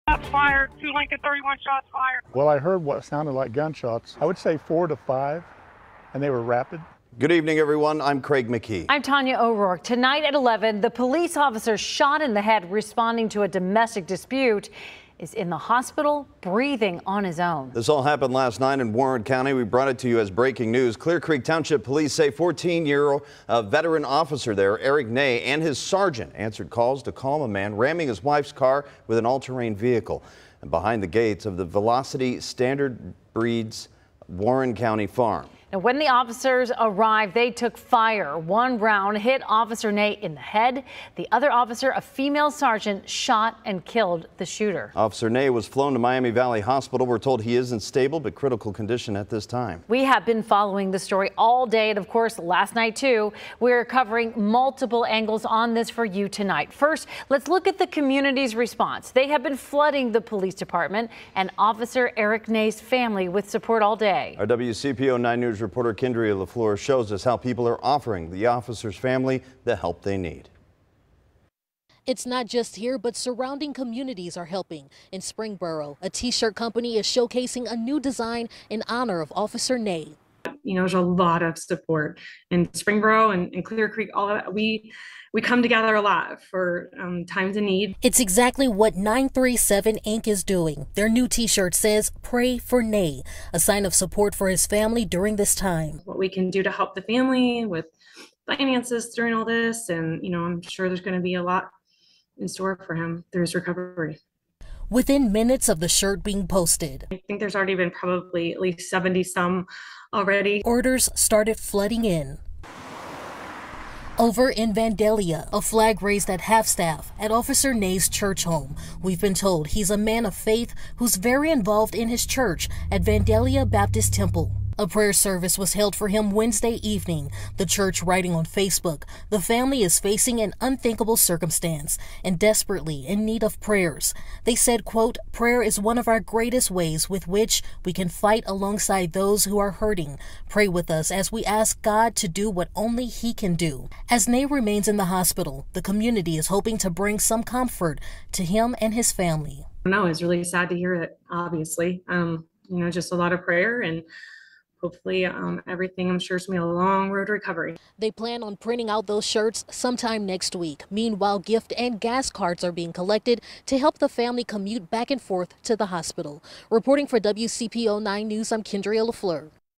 Community rallies behind Clearcreek police after officer shot
A flag is raised at half-staffed at Officer Eric Ney's church, where members held a prayer service for the officer shot while responding to a domestic incident in Clearcreek Township.